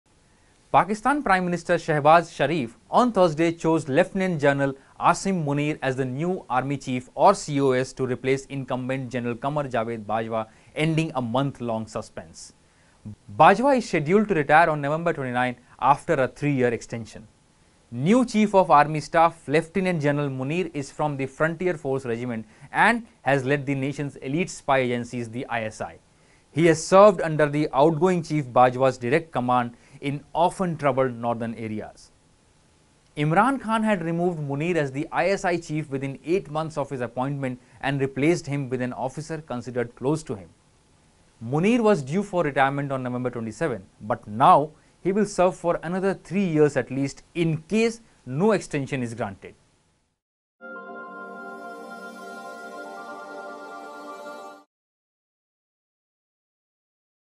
Asim Munir: Pakistan's former ISI chief named as army head
After a month of suspense, Pakistan has finally announced it's new army chief. Lt Gen Asim Munir willl replace the incumbent General Qamar Javed Bajwa. Bajwa, 61, is scheduled to retire on November 29 after a three-year extension. He had ruled out seeking another extension. Lt Gen Sahir Shamshad Mirza had been picked as the Chairman of the Joint Chiefs of Staff Committee (CJCSC). Both officers have also been promoted to four-star generals.